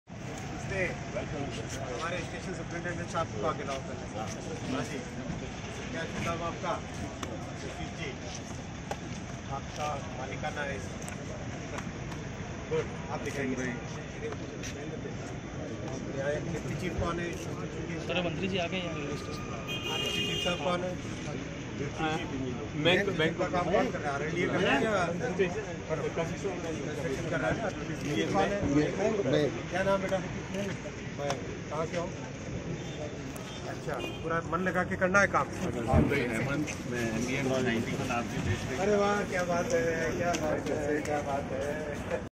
श्री अश्विनी वैष्णव, माननीय रेल, संचार, इलेक्ट्रॉनिक्स और सूचना प्रौद्योगिकी मंत्री का उदयपुर दौरा
श्री अश्विनी वैष्णव, माननीय रेल, संचार, इलेक्ट्रॉनिक्स और सूचना प्रौद्योगिकी मंत्री का उदयपुर दौरा। स्टेशन सुपरिटेंडेंट एवं अन्य अधिकारियों से किया संवाद